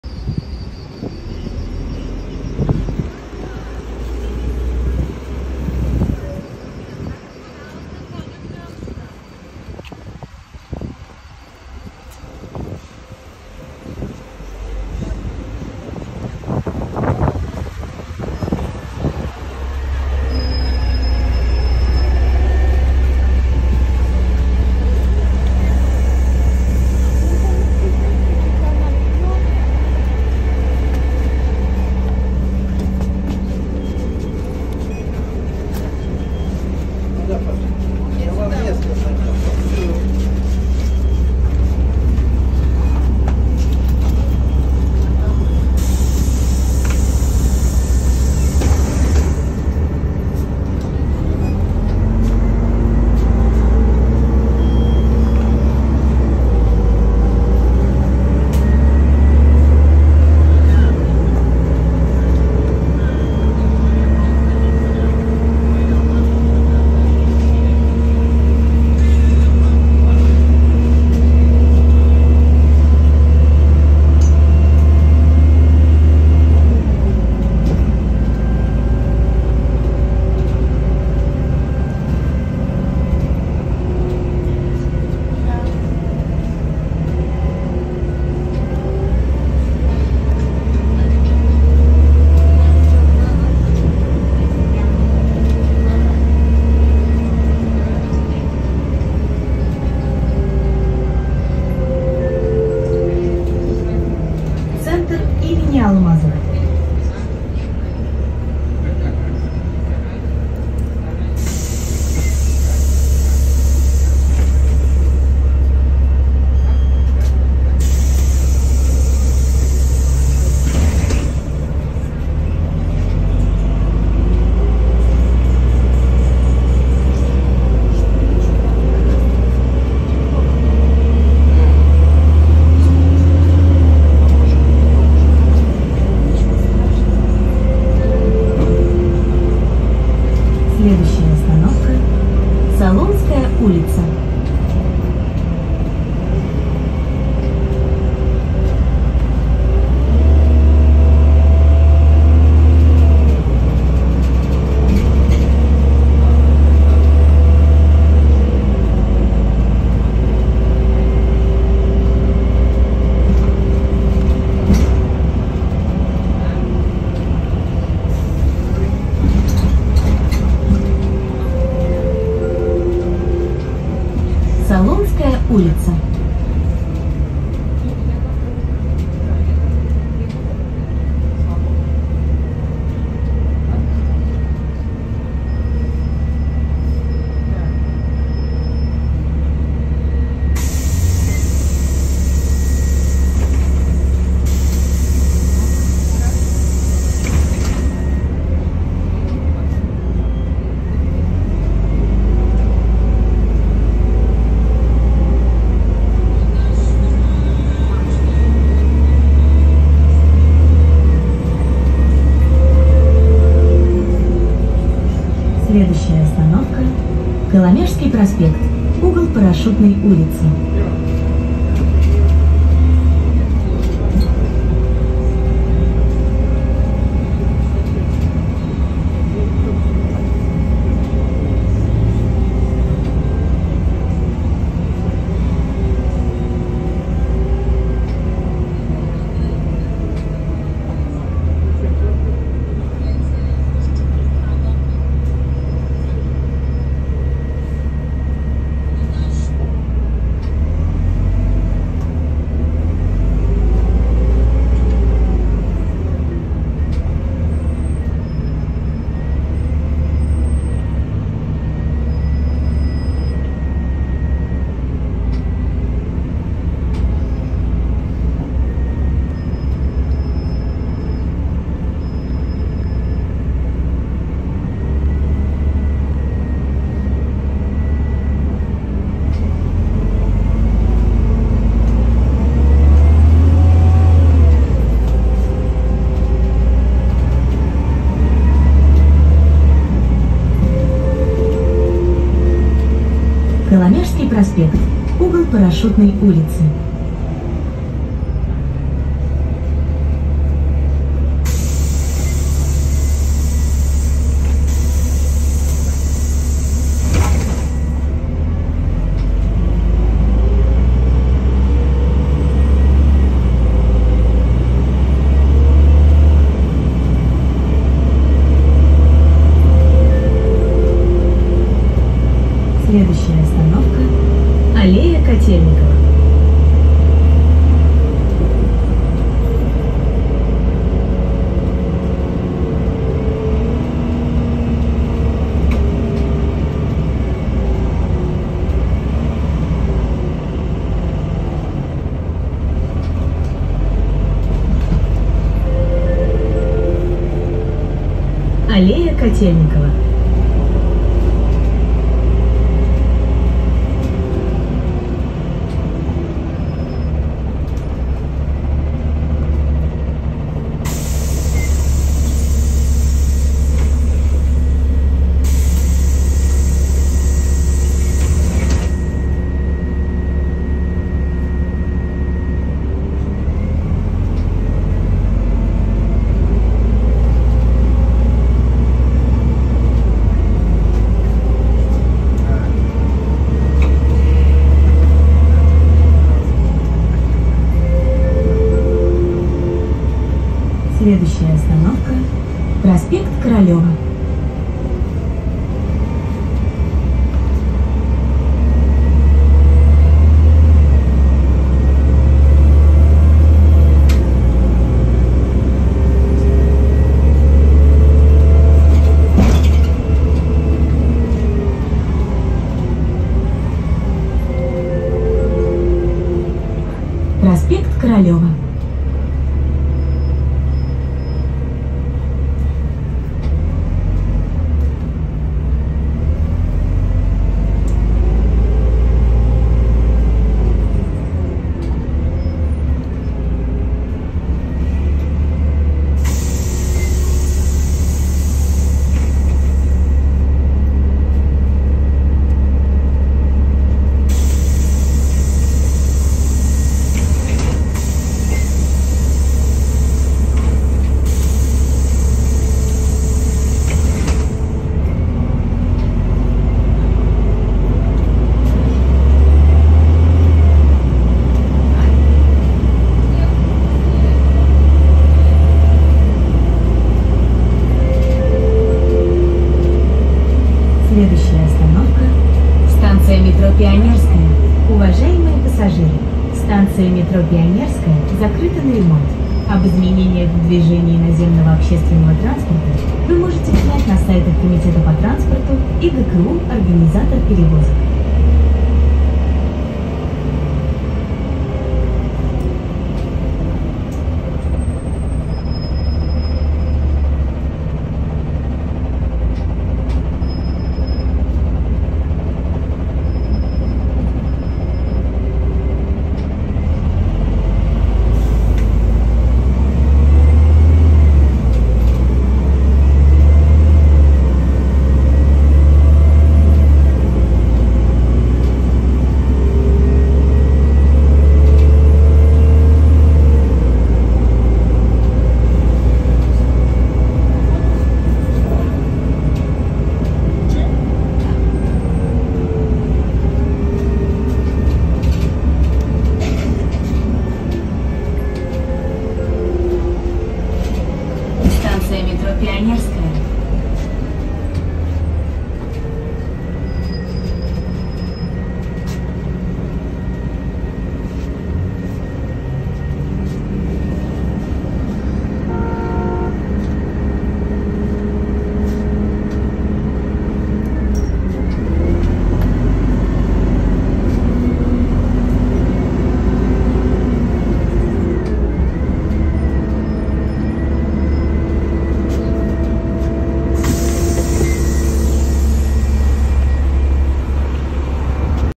«От Удельной до Пионерской на автобусе по 279» Поездка на автобусе Нефаз-5299 по маршруту 279
Поездка от остановки Ж/д ст. Удельная до остановки Ст.м Пионерская